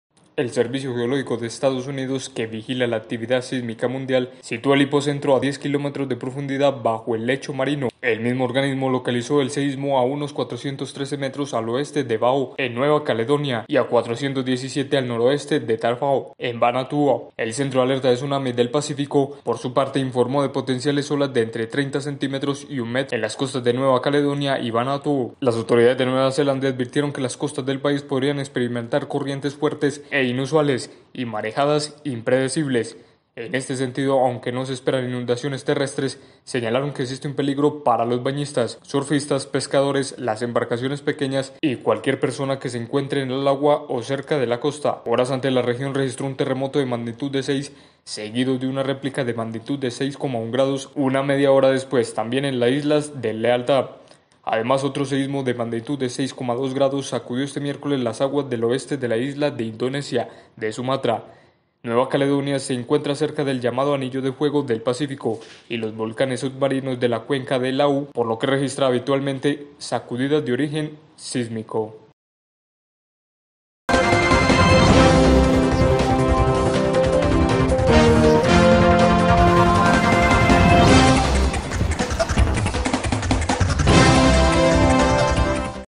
Sismo de 7.1 sacudió el pacifico sur
Sismo de 7,1 grados sacudió el pacifico sur generando alerta de Tsunami en Nueva Zelanda y Nueva Caledonia.